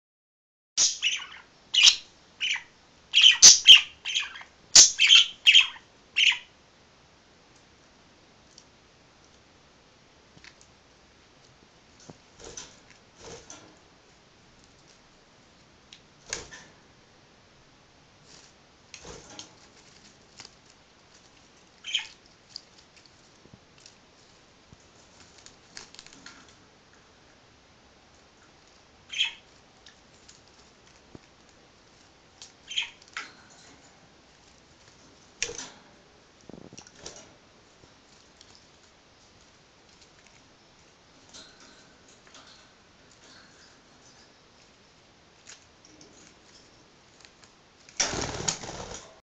Budgie taking bath in a kasten
Some Budgies are so cute that you want to eat them
 and some budgies are so cute that you want them to take a bath.